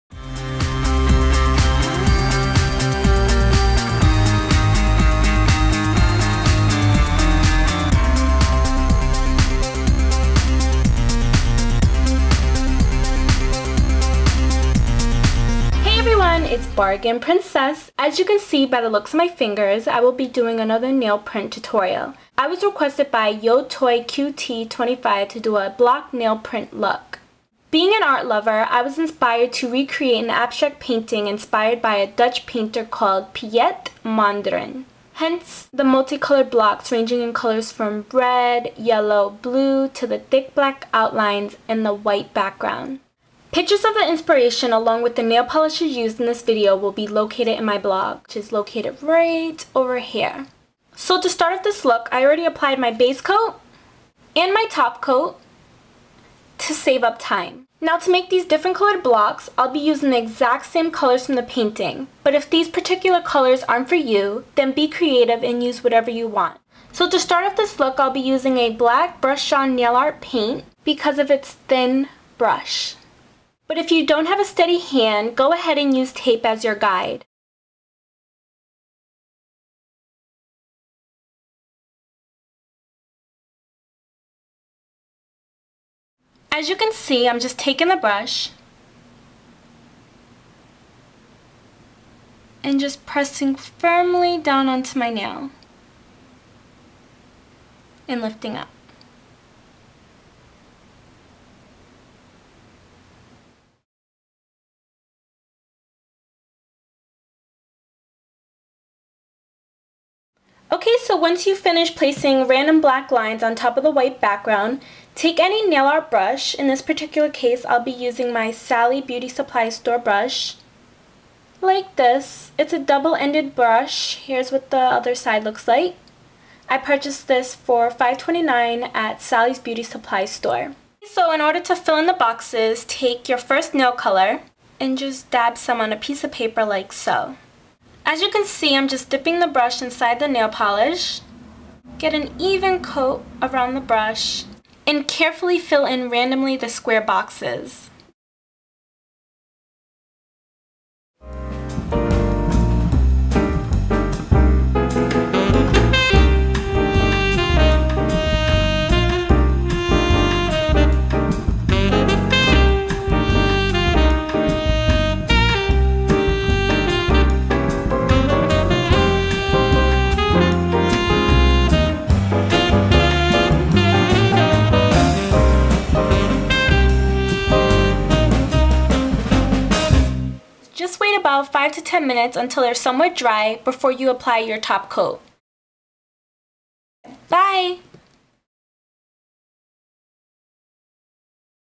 Block Nails!! Inspired By Dutch Painter Piet Mondrian
After I uploaded my back to school makeup tutorial this has now been a highly requested video by many of you!! So I hope you all enjoy :D